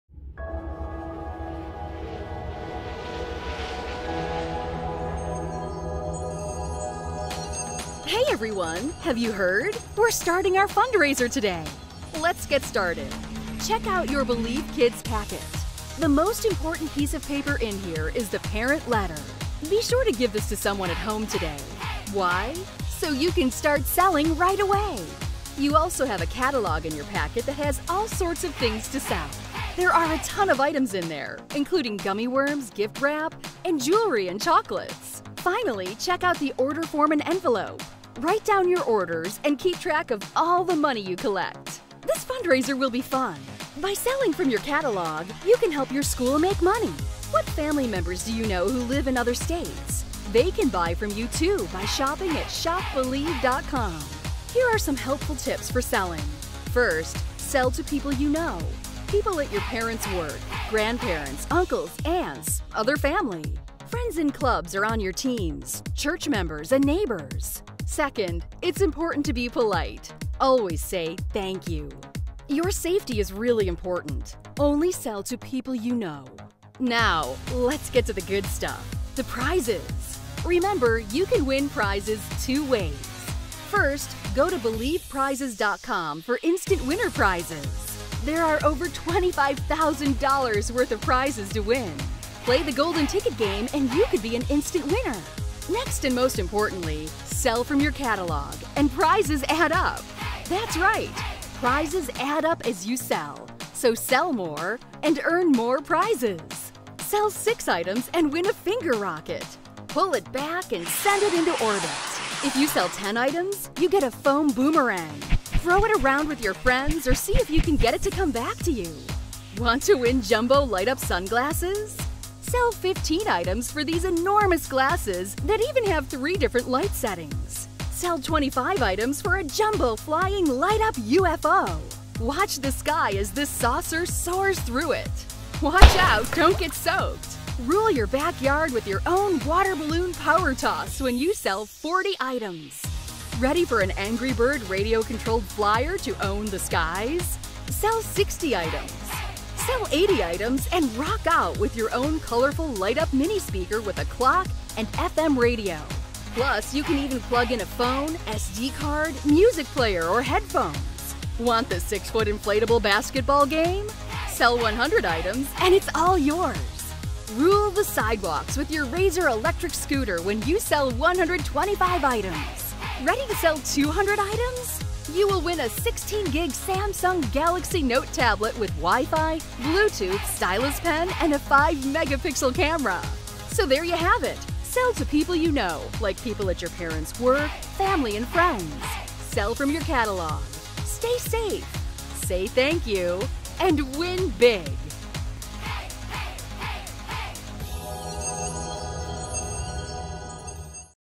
2014 Believe Kids Kickoff Video for Elementary Schools Prize City selling Catalog
Fundraising kickoff video from Believe Kids Elementary School Fundraising. This video is for support during your live kickoff or in cases where schools are not able to do a live fundraising kickoff.